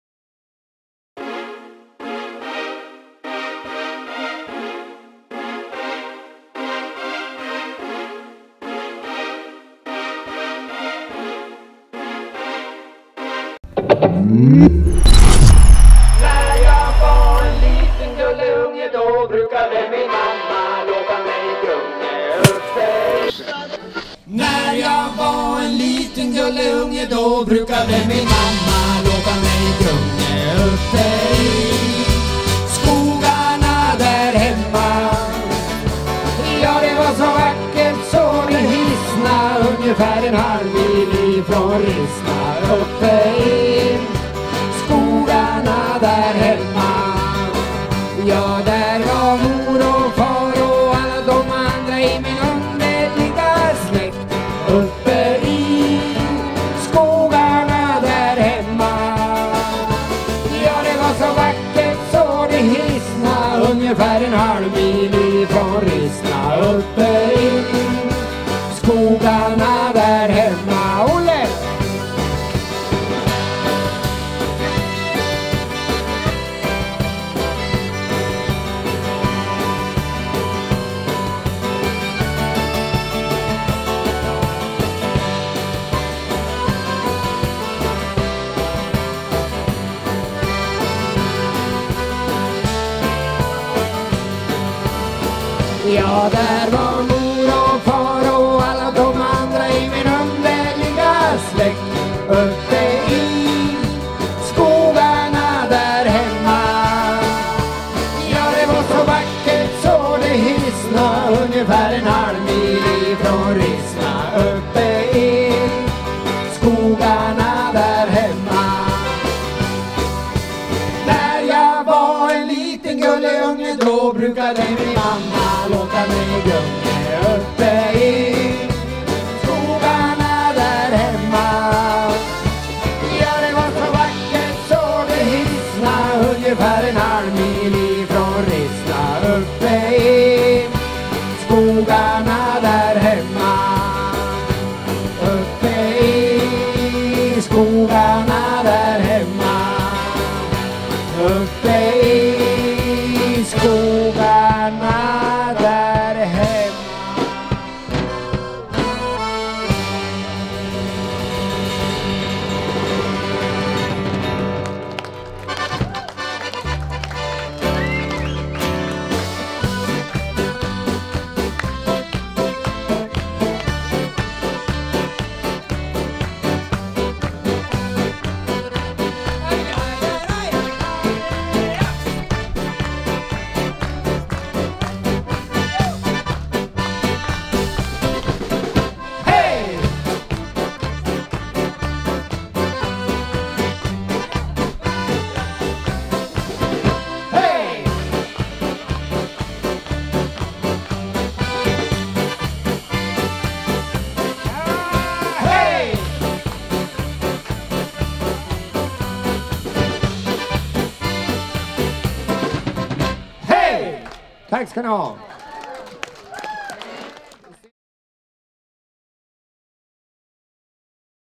Åsgård Band -Rissnadagarna 2018
Åsgård Band från Åsgård Jämtland
Rissnadagarna 2018
Kontrabas Hasse Lindqvist
Dragspel Olle Lindqvist
Gitarr Marcus Ekeljung
Trummor Mr Lindqvist
Åsgård, Jämtland
Musik
MUSIK & TEXT 2018©️ Åsgård Band
FOTO/VIDEO: 2018©️ Joachim Lechte & Wilmer Fälldin